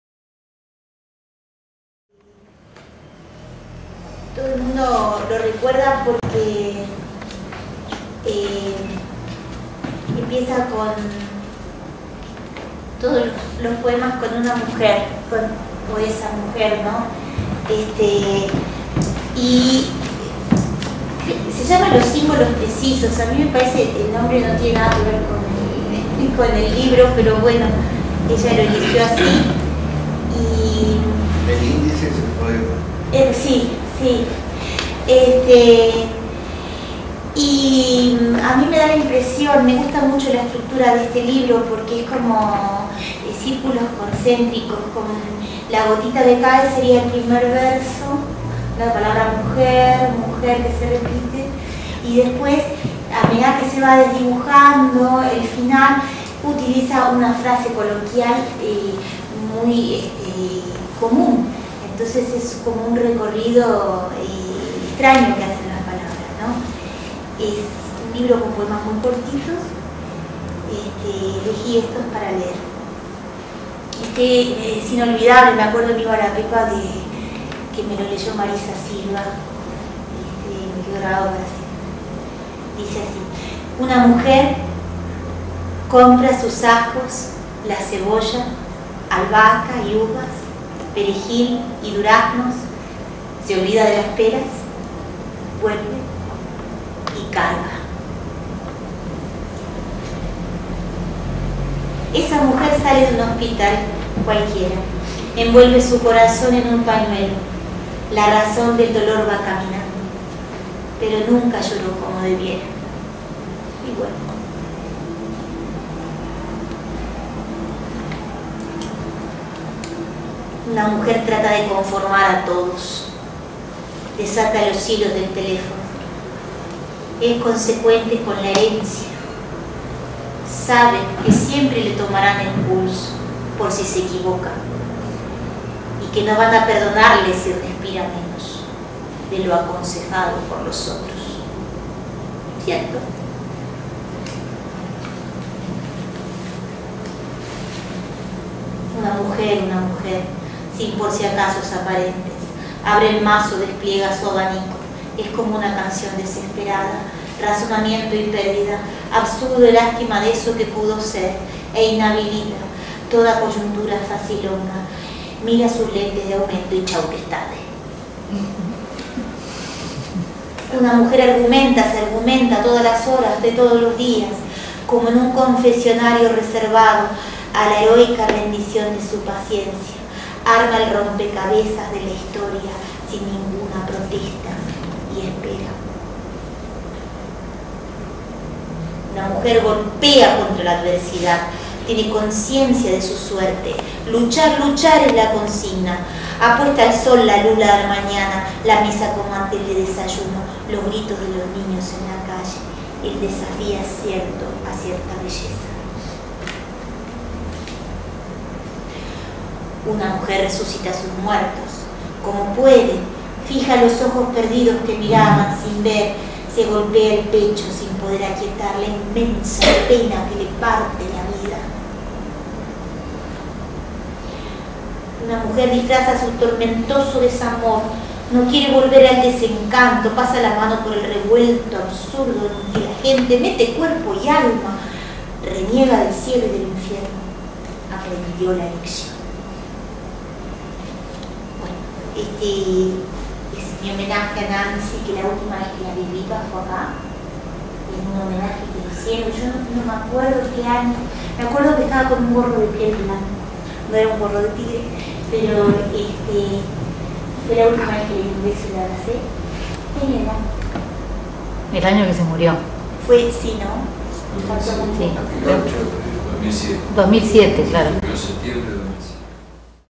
Andrea Blanqué: homenaje a Nancy Baceló
Ciclo de poesía "La punta de la lengua"
Montevideo junio 2013
Academia Nacional de Letras
Casa de Julio Herrera y Reissig